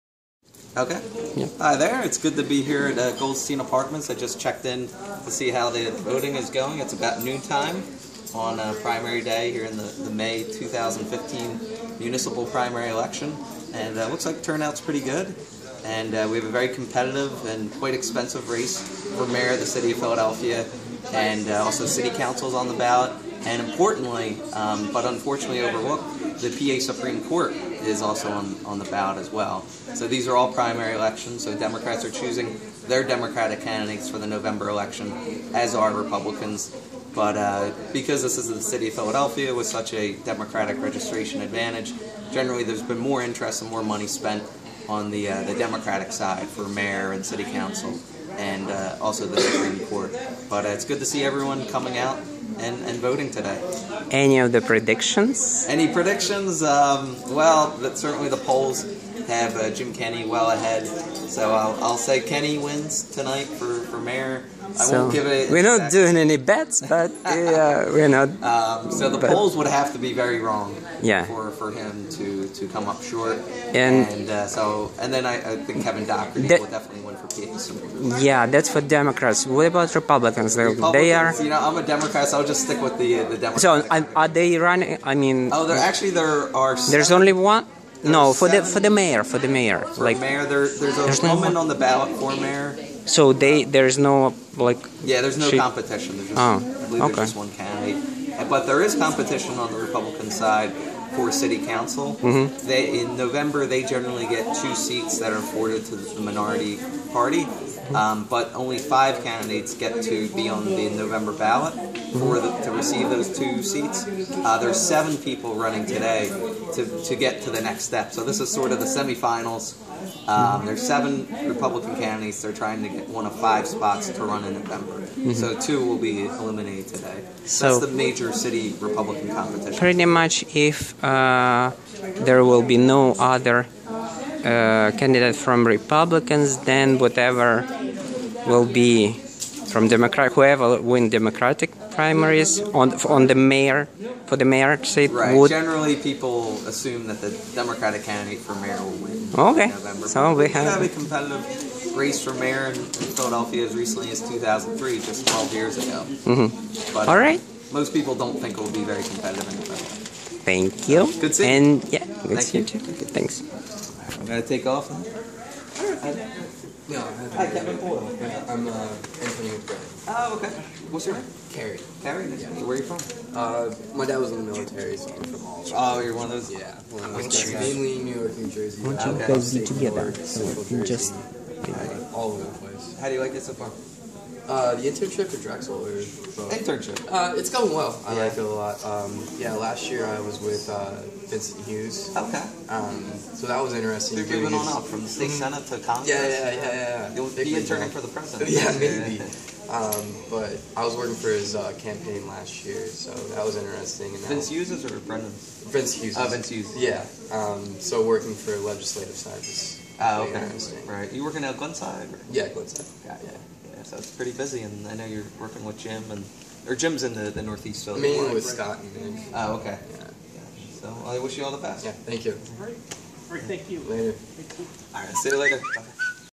Short Interview with Kevin Boyle at the pole of Philly Primary Election 2015
Recorded with phone...